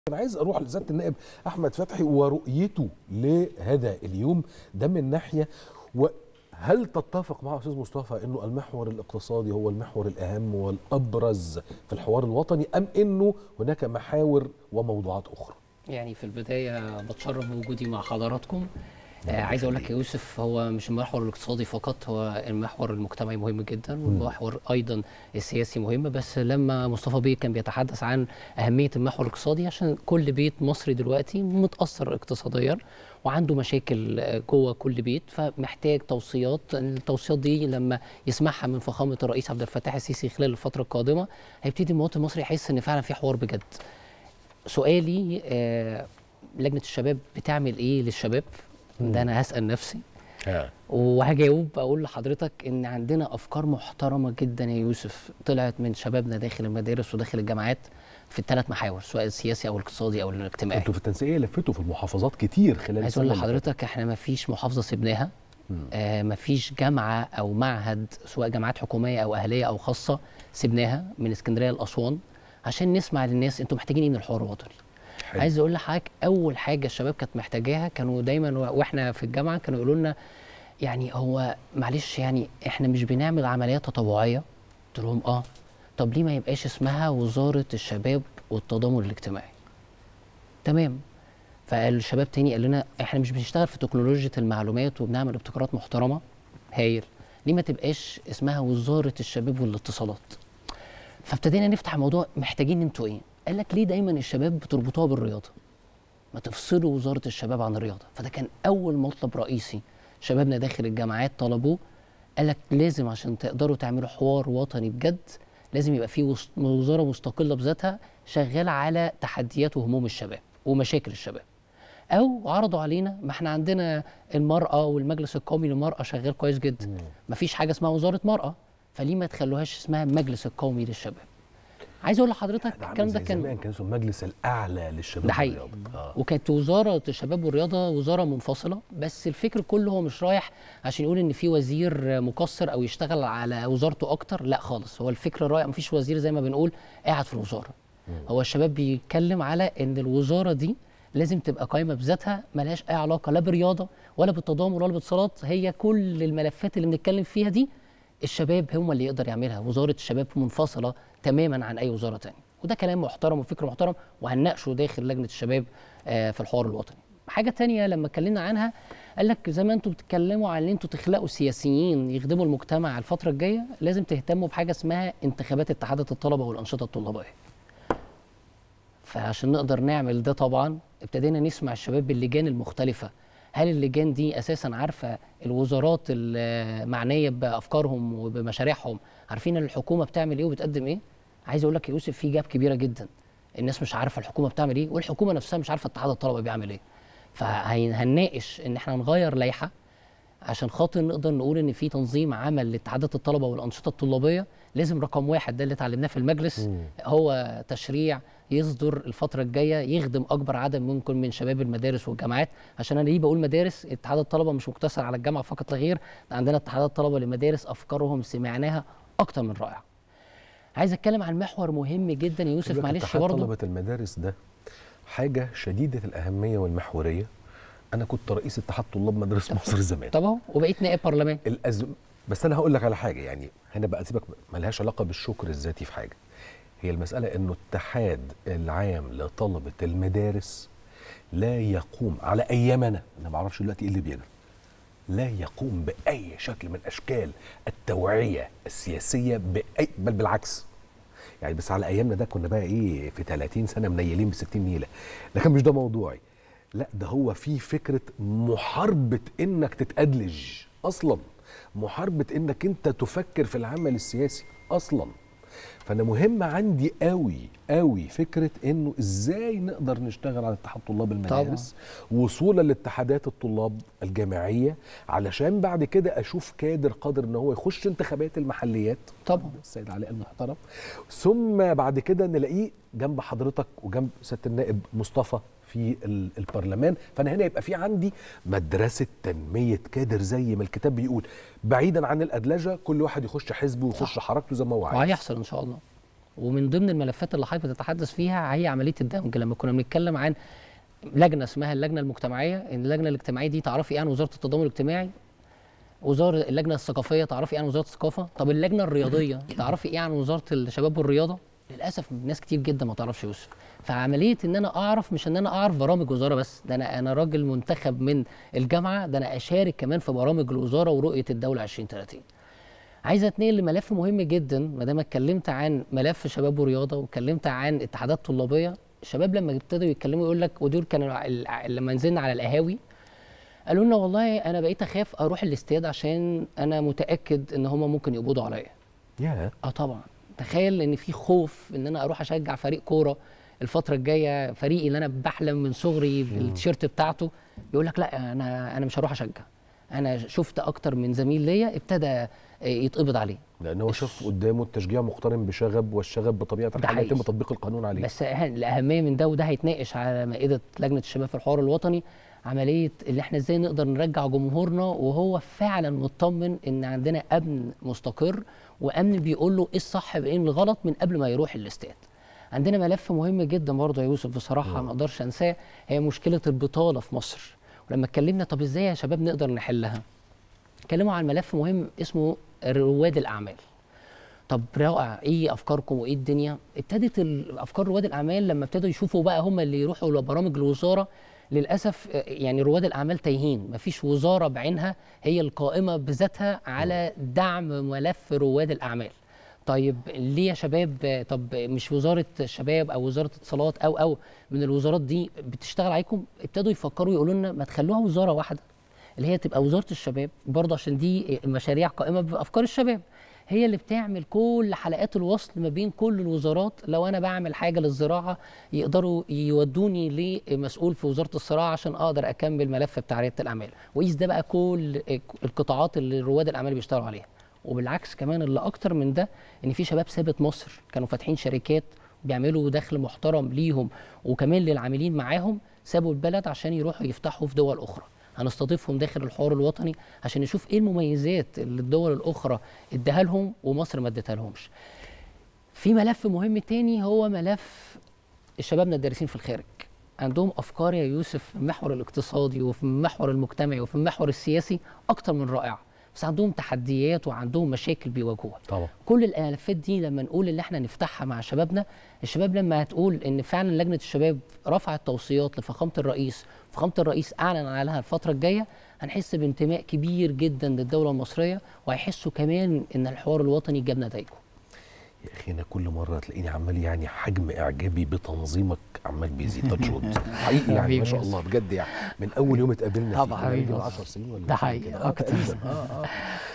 النائب أحمد فتحي عضو تنسيقية شباب الأحزاب: مفيش محافظة إلا وسألنا أهلها محتاجين إيه من الحوار الوطني
التاسعة | النائب أحمد فتحي عضو مجلس النواب عن تنسيقية شباب الأحزاب: مفيش محافظة إلا وسألنا أهلها محتاجين إيه من الحوار الوطني